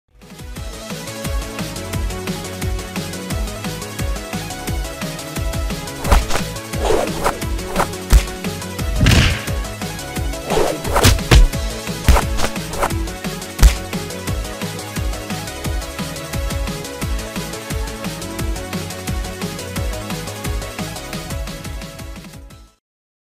[SFM] Sparring Practice
Been inspired a lot by my friend Shadow759 and fight videos that moved me to want to do this, I've always wanted to do a nice hand to hand combat type of animation. I hope to be able to apply what I learned here in future videos, this was a blast to do but boy it took some time for each basic move XD I hope you all enjoy this brief very brief animation and a huge thanks to my best friend Flora-Tea for Hitfilming this video!

Freedom Planet models provided by Disaccord Noob

Freedom Planet characters belong to Galaxy Trail.

Animation by me using Source Filmmaker from Valve.